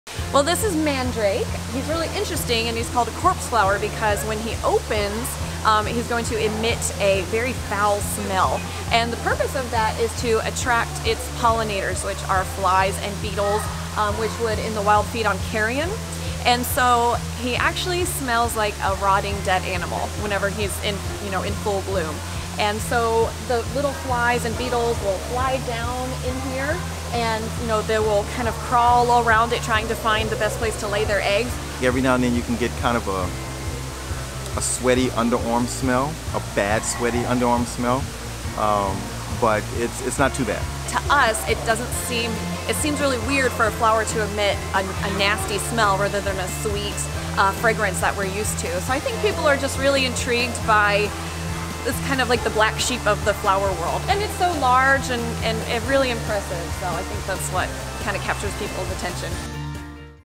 Stinky, Rare Corpse Flower Blooms in Houston Museum
Mandrake, the stinky corpse flower, is now blooming at the Houston Museum of Natural Science.

The museum said the plant is now in bloom and smells like "death."

The stinky flower will be on exhibit starting this weekend.

Known as amorphophallus paeoniifolius, the plant is originally found in Southeast Asia and Australia. It is in the Aroid family and is characterized by its inflorescence, consisting of a fleshy spike of small flowers.

While it takes most corpse flowers between seven and 10 years to bloom, once they do, the bloom lasts for just 24 to 36 hours.